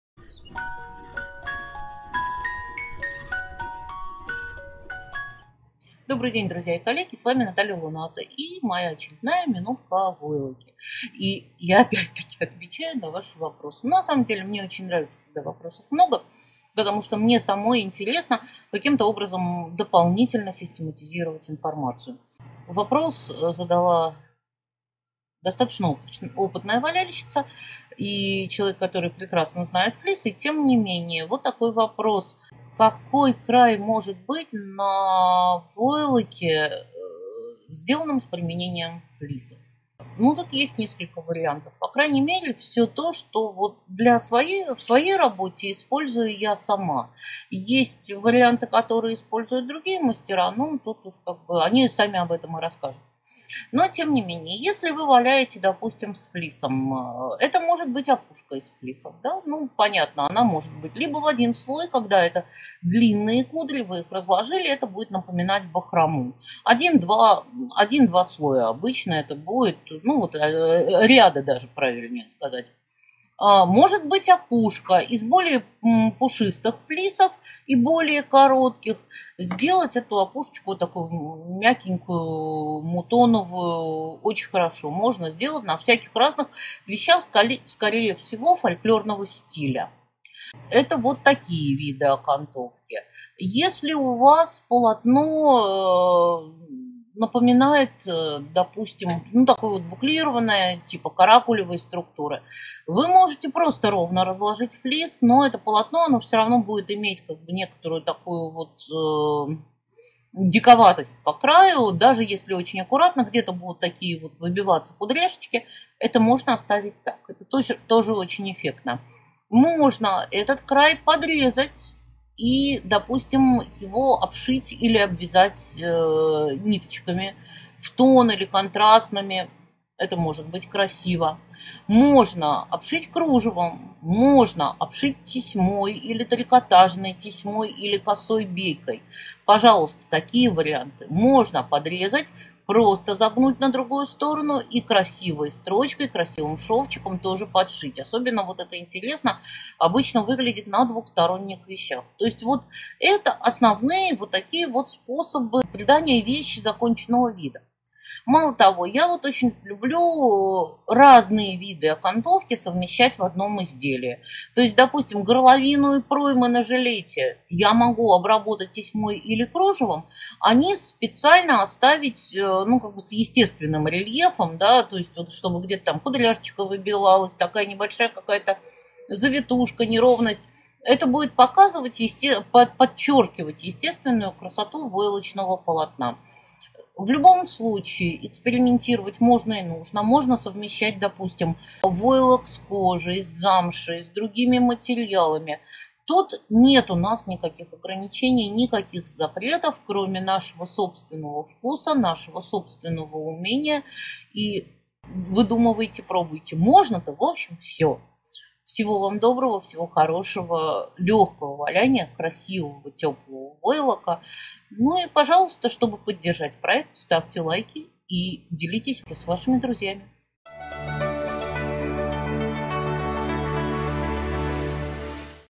Как отделать края войлока из флисов? Минутка о войлоке
Продумывая дизайн войлочной одежды из флисов, можно использовать разные способы отделки края изделий.
Какие способы отделки наиболее применимы и популярны?
Что лучше - бахрома, меховая опушка, окантовка или подгиб?

Отвечаю на вопросы коллеги в новой минутке о войлоке.